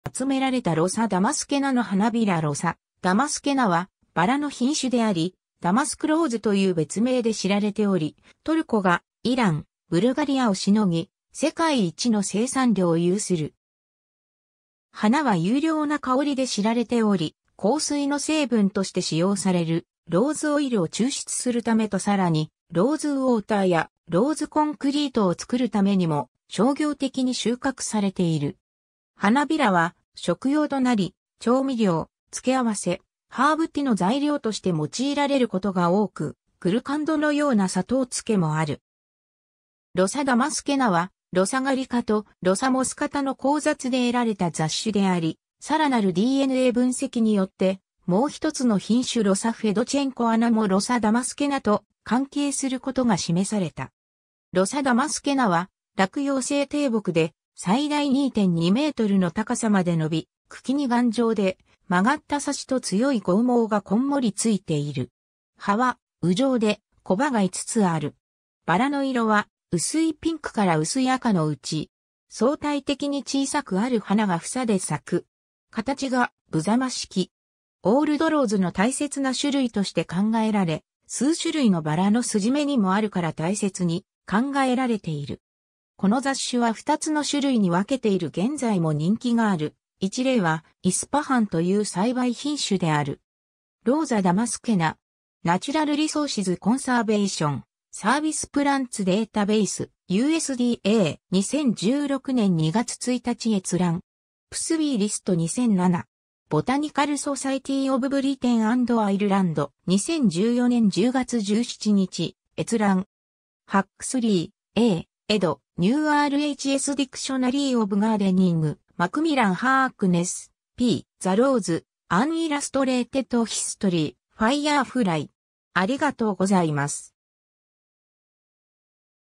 ロサ・ダマスケナ
ジャムの材料として集められたロサ・ダマスケナの花びら ロサ・ダマスケナ（Rosa × damascena）は、バラの品種であり、ダマスクローズ（Damask rose）という別名で知られており、トルコがイラン、ブルガリアを凌ぎ、世界一の生産量を有する。
 花は優良な香りで知られており、香水の成分として使用されるローズオイル（ローズ・オットー又はローズ・アブソリュート）を抽出するためとさらにローズウオーターやローズコンクリートを作るためにも商業的に収穫されている。
 花びらは食用となり、調味料、付け合わせ、ハーブティの材料として用いられることが多く、グルカンドのような砂糖漬けもある。
 ロサ・ダマスケナはロサ・ガリカ(Rosa gallica)とロサ・モスカタ(Rosa moschata)の交雑で得られた雑種であり、さらなるDNA分析によって、もう一つの品種ロサ・フェドチェンコアナもロサ・ダマスケナと関係することが示された。
 ロサ・ダマスケナは落葉性低木で、最大2.2mの高さまで延び、茎に頑丈で曲がった刺と強い剛毛がこんもりついている。
葉は羽状で小葉が5つ（珍しくは7つ）ある。
バラの色は薄いピンクから薄い赤の内。
相対的に小さくある花が房で咲く。
形が不様式。
オールドローズの大切な種類として考えられ、数種類のバラの筋目にもあるから大切に考えられている。
 この雑種は2つの種類に分けている 現在も人気がある一例はイスパハンと言う栽培品種である。
 ^ "Rosa ×damascena". Natural Resources Conservation Service PLANTS Database. USDA. 2016年2月1日閲覧。
 ^ “BSBI List 2007 (xls)”. Botanical Society of Britain and Ireland. 2014年10月17日閲覧。
 ^ Huxley, A., ed (1992). New RHS Dictionary of Gardening. Macmillan ^ *Harkness, P. (2003). The Rose: An Illustrated History. Firefly